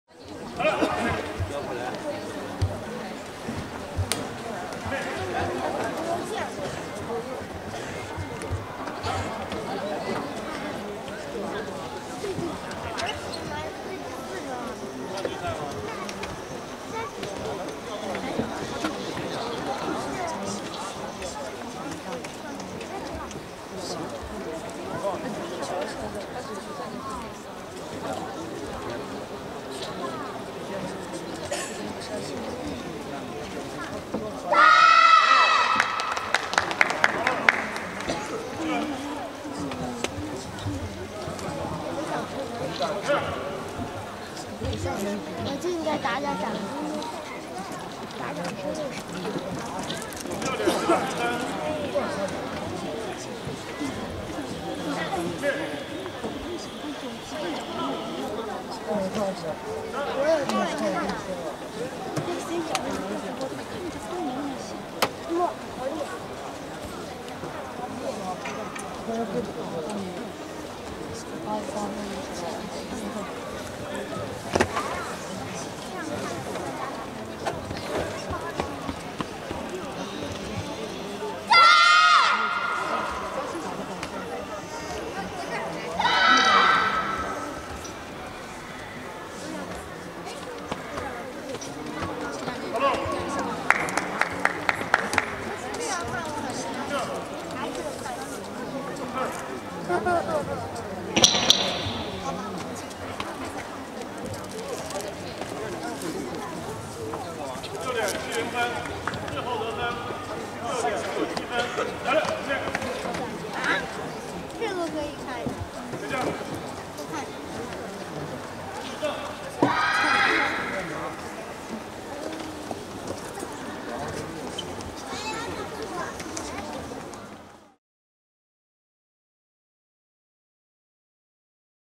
2017 China Open Group Poomsae，Kids 2nd，2017 中国跆拳道公开赛 团体品势 儿童组 第2名 浙江跆协三队 韩伯韬 宋毅昊 徐俊毅
2017 China Open Group Poomsae，Kids 2nd
02 2017 中国跆拳道公开赛 团体品势 儿童组 第2名 浙江跆协三队 韩伯韬 宋毅昊 徐俊毅